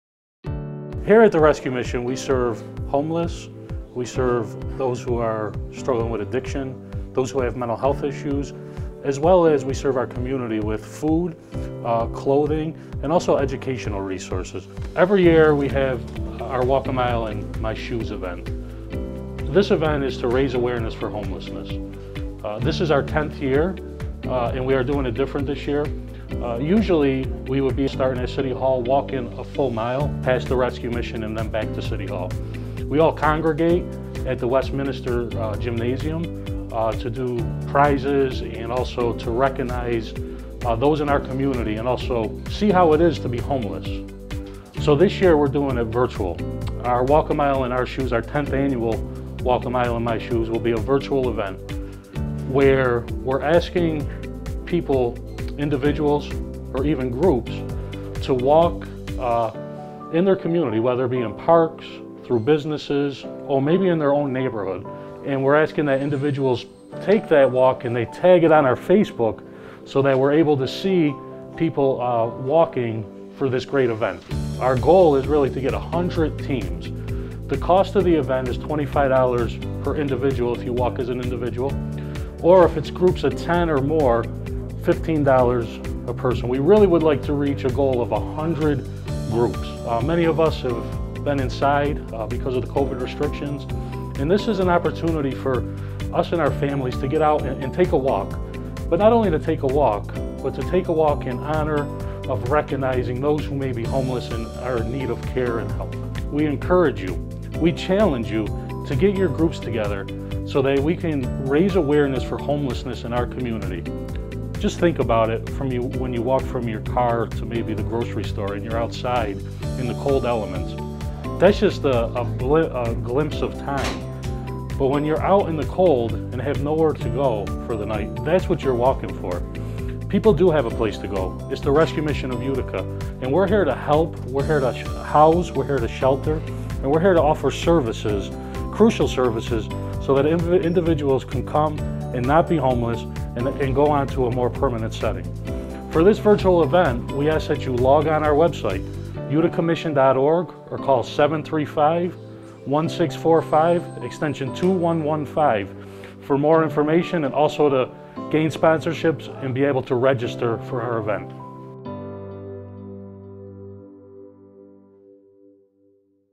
Walk A Mile In My Shoes Utica 2021
The Rescue Mission of Utica is hosting a virtual Walk A Mile In My Shoes this year! You can choose to “Walk a Mile in My Shoes” any day in February, at the time and place of your choice, all to raise money for the Rescue Mission services in battling homelessness in our community.